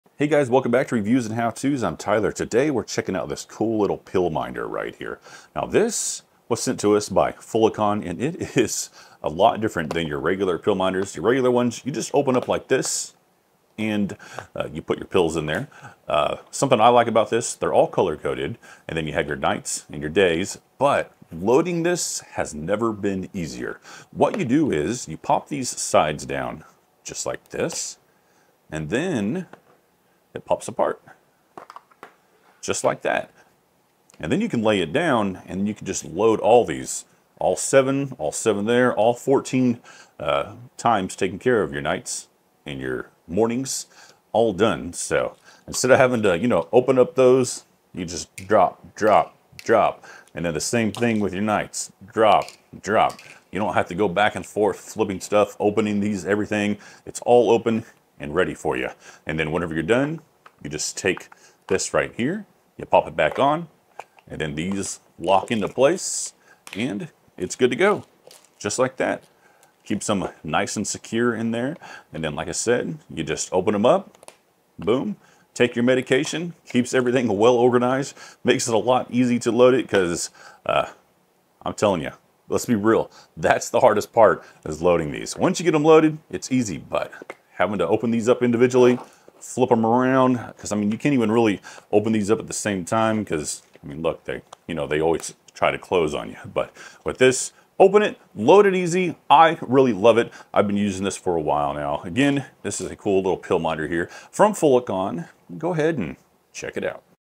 Fullicon Quick Fill Pill Organizer Review: The Best Weekly Pill Box!
The Fullicon Easy Fill Pill Organizer offers a large capacity with 14 compartments, each holding up to 13 fish oils or 12 vitamins, making it perfect for managing your daily supplements. Made from BPA-free, food-grade plastic, this pill organizer features a detachable base for quick filling and secure side buckle locks to keep your pills protected.

*Check Today's Deal on the Fullicon Pill Organizer on Amazon:* (ad)

*Interested in having me conduct a product review or Amazon shoppable video?*